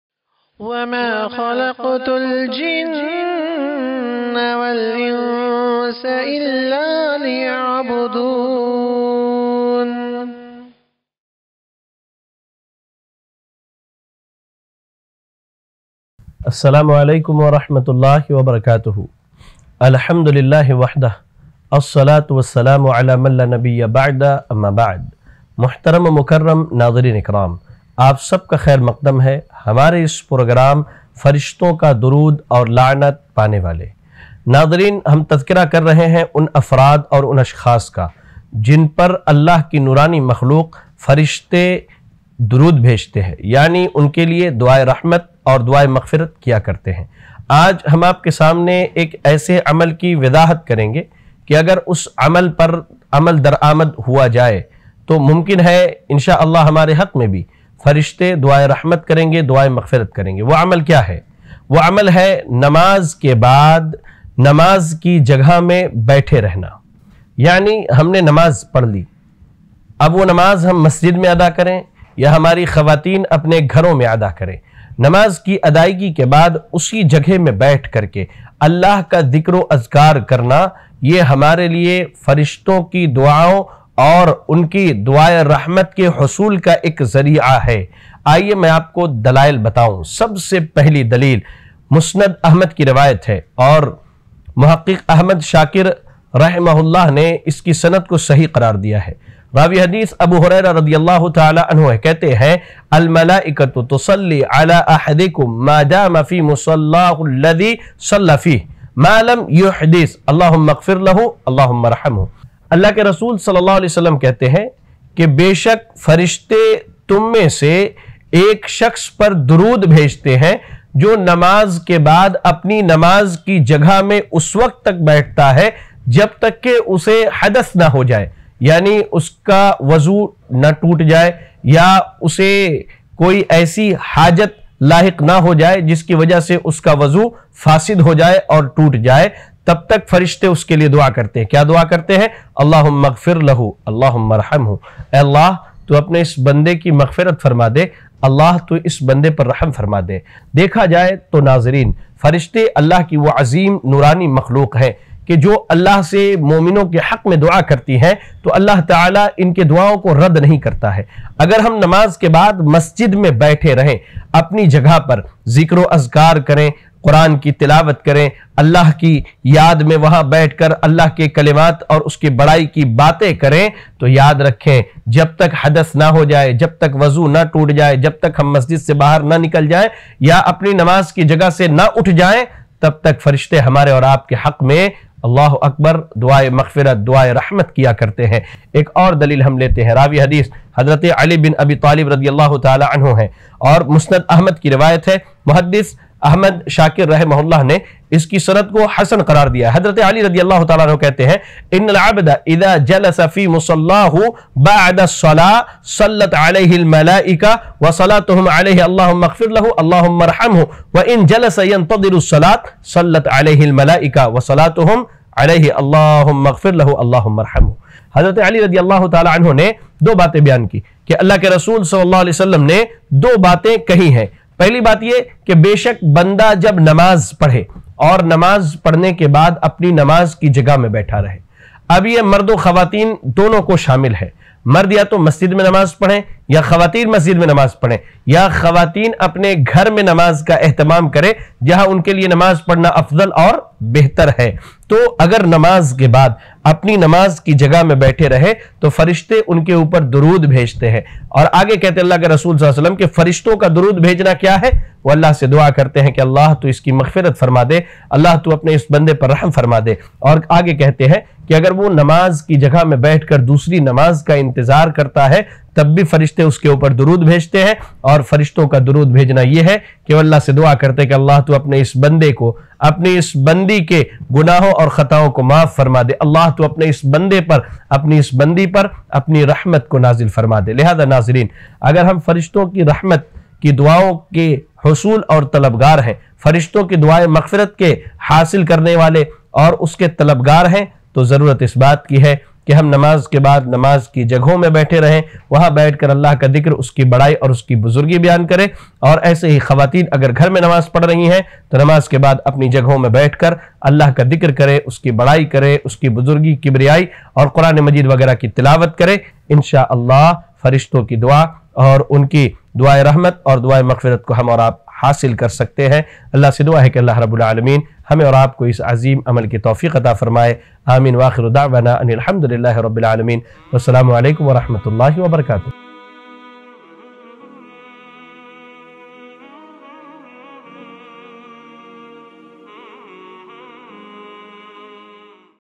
Namaz ke bad usi jagah baithne ki fazilat | Farishton Ka Darood Aur Lanat Paane Wale 07 | Sk Imran
Namaz ke bad usi jagah baithne ki fazilat | Farishton Ka Darood Aur Lanat Paane Wale 07 | Shaikh Imran Ahmed Taifi At Tibyaan TV